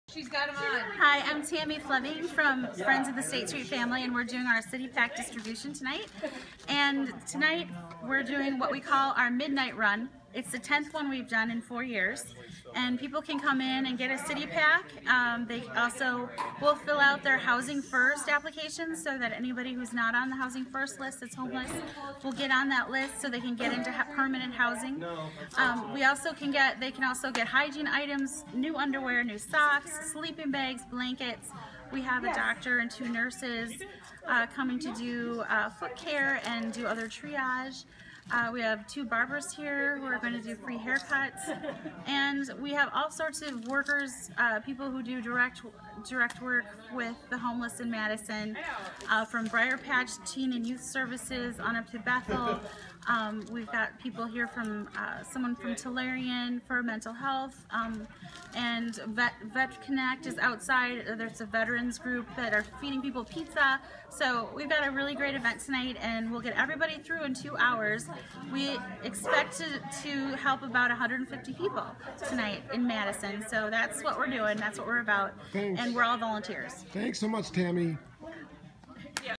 Friends of the State Street Family X CITYPAK - Partner Testimonial
Friends of the State Street Family's mission is to alleviate human suffering by providing help and hope to people experiencing homelessness by connecting them with essential resources. Our partners often utilize CITYPAKs as a starting point for creating discussion with those in their communities who could benefit from the organizations support, and we're proud to be a part of that story.